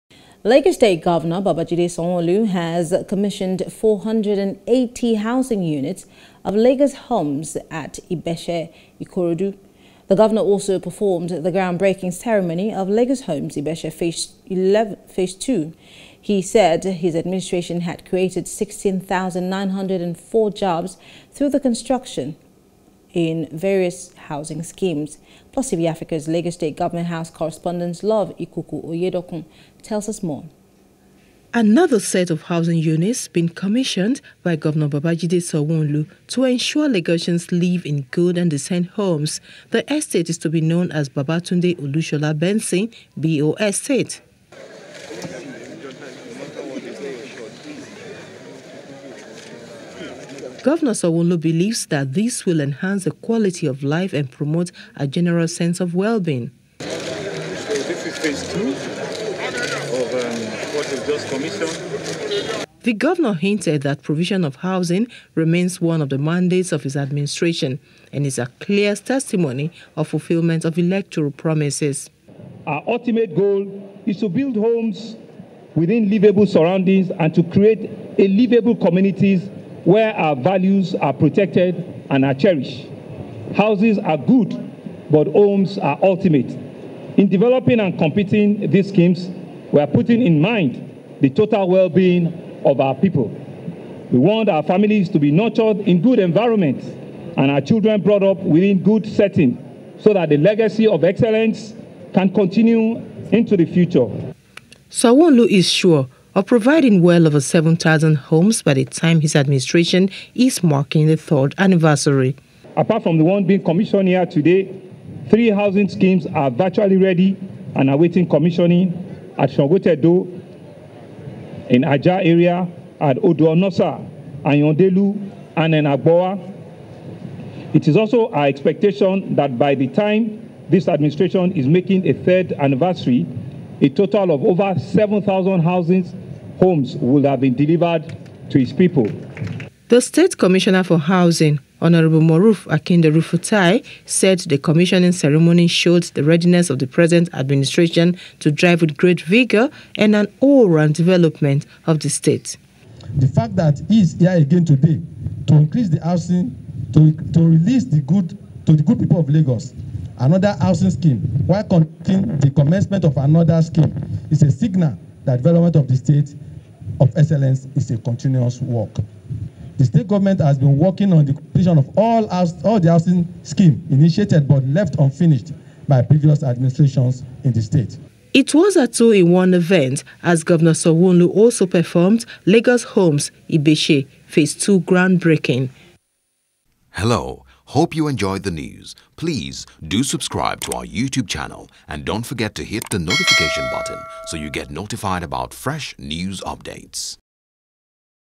Housing: Gov. Sanwo-Olu Commissions 480 Units At Ibeshe | NEWS
Lagos State Governor Babajide Sanwo-Olu has commissioned 480 Housing Units of LagosHOMS at Ibeshe, Ikorodu.